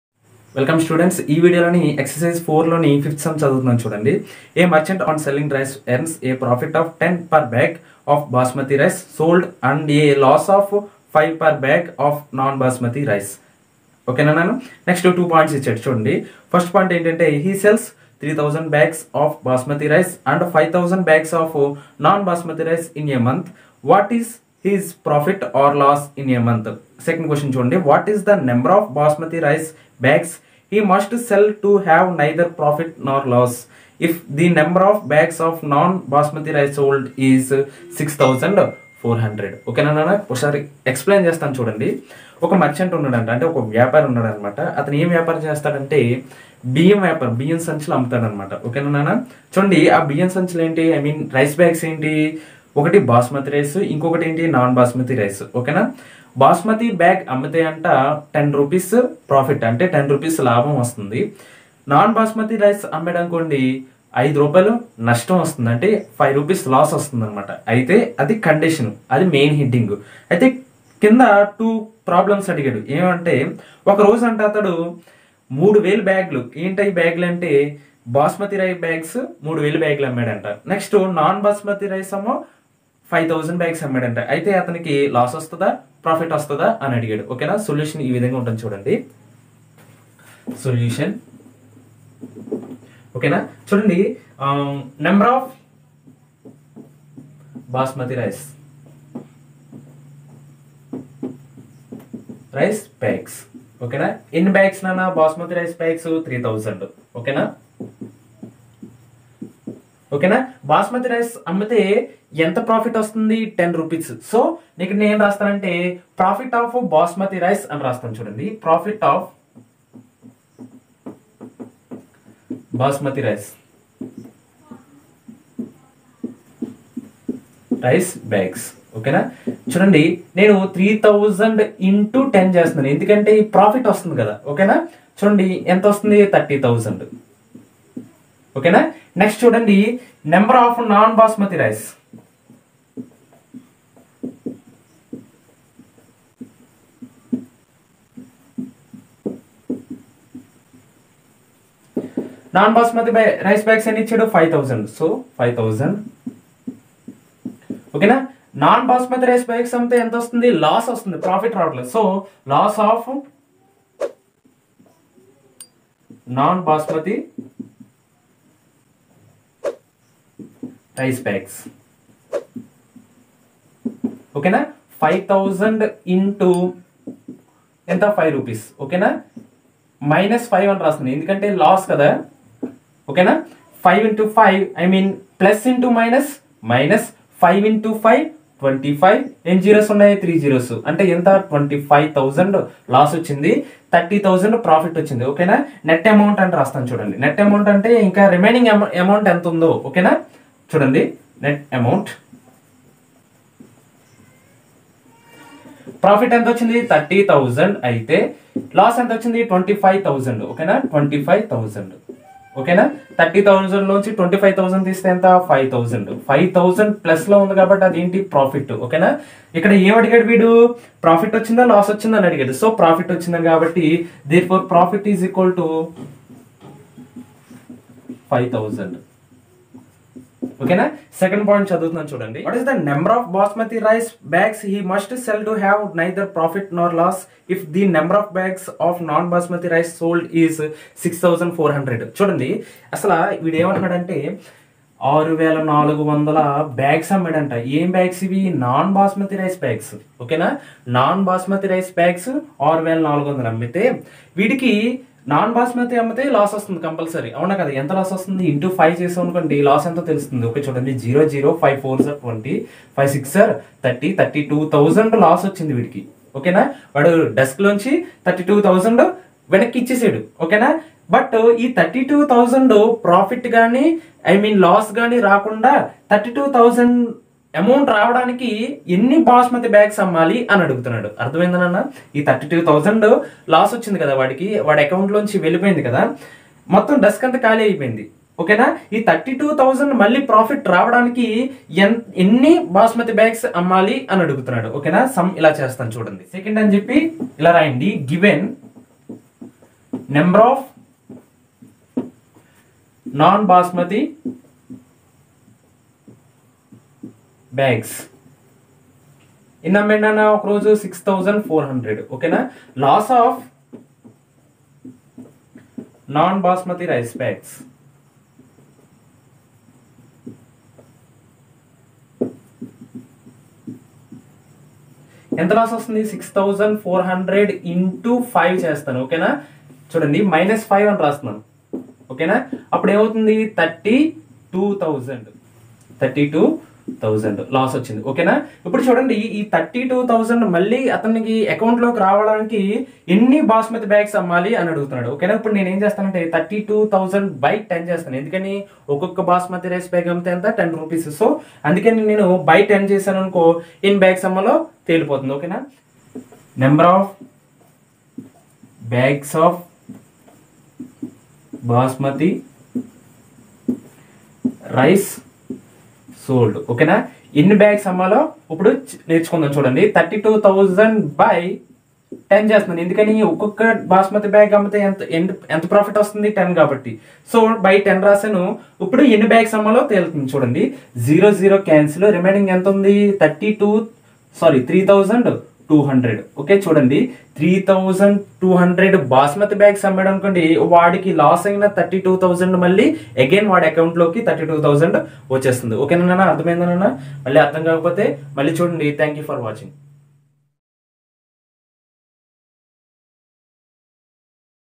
A merchant on selling rice earns a profit of `10 per bag of basmati rice sold and a loss of
5) A merchant on selling rice earns a profit of  `10 per bag of basmati rice sold and a loss of  `5     per bag of non-basmati rice.
(i) He sells 3,000 bags of basmati rice and 5,000 bags of non-basmati rice in a month. What is his     profit or loss in a month?
(ii) What is the number of basmati rice bags he must sell to have neither profit nor loss, if the   number of bags of non-basmati rice sold is 6,400.

Mic:
Generic collar mic clip with long cable for mobile,dslr camera,etc (black)

tq